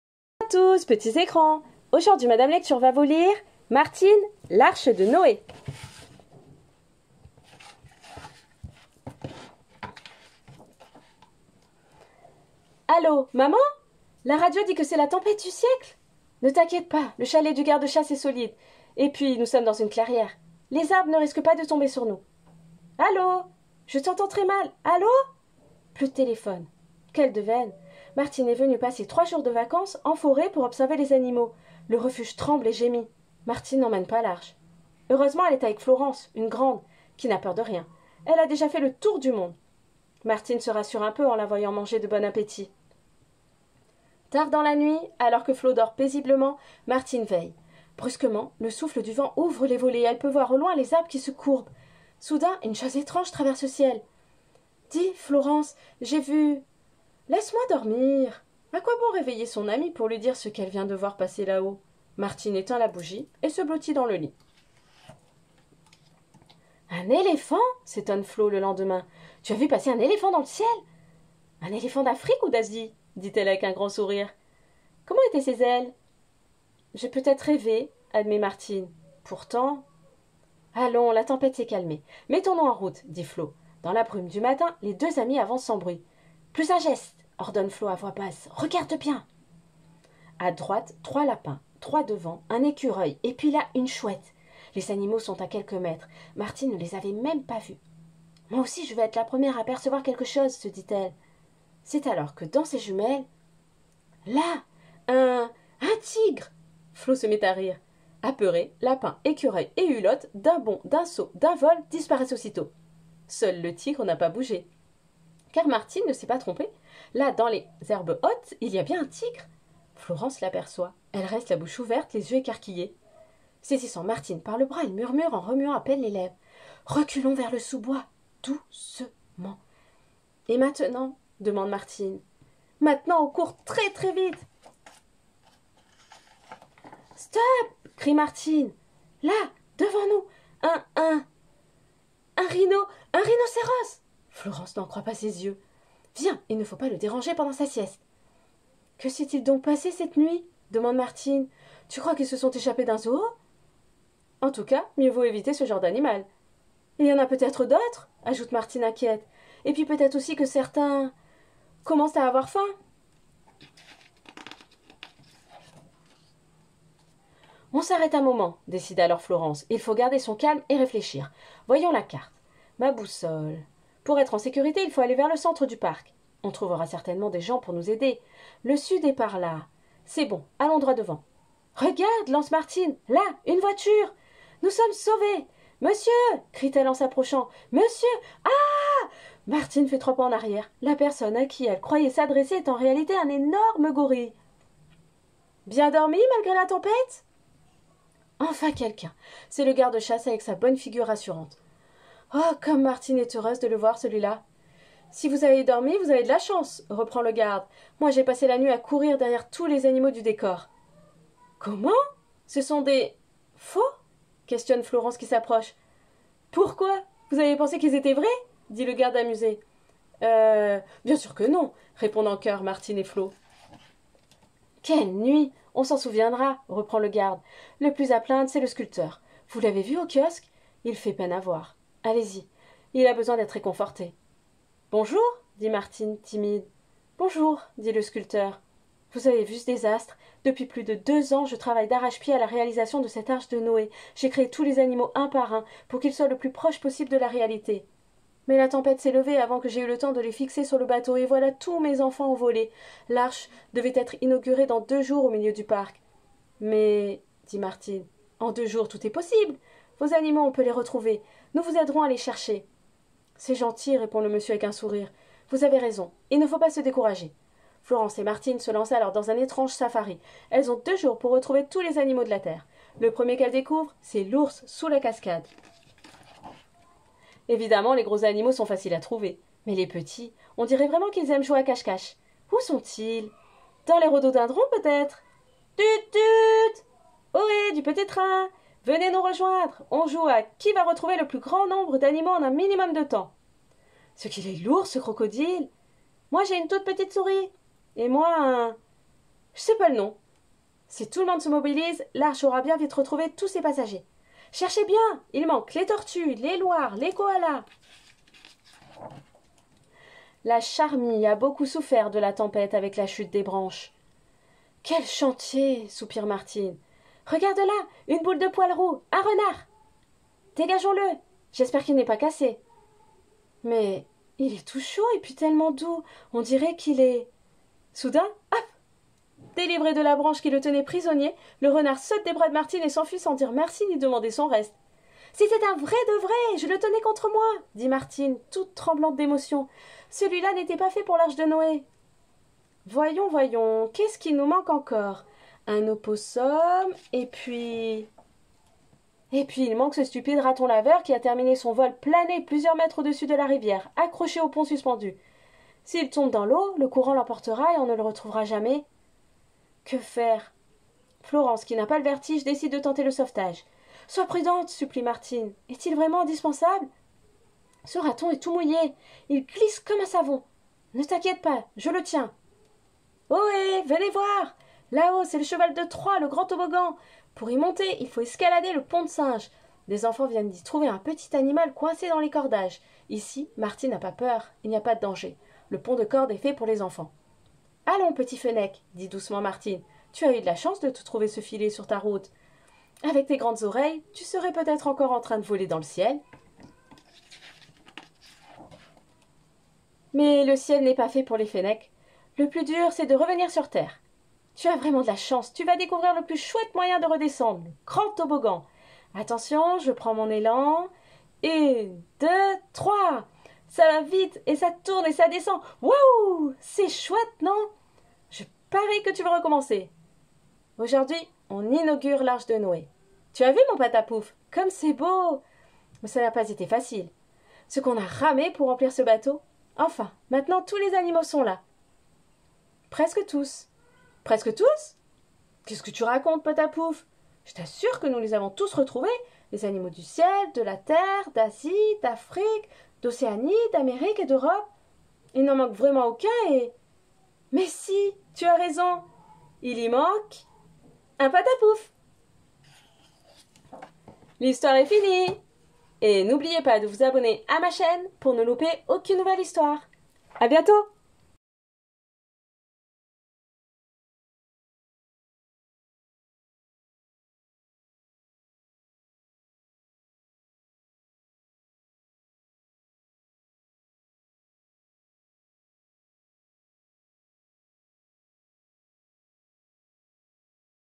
C'est l'heure d'écouter Martine L'arche de Noé 🦒
L'histoire de Martine L'arche de Noé est la suivante.
Avec son amie ornithologue, Martine passe le week-end dans un parc naturel. Elles pensent avoir la possibilité d'observer les oiseaux. Or, un violent orage éclate pendant la nuit et les arbres sont arrachés. Le lendemain, elles découvrent que le parc est dévasté ainsi que de bien curieux oiseaux... Que font là, ces singes, ces lions, ces girafes et ces ours ?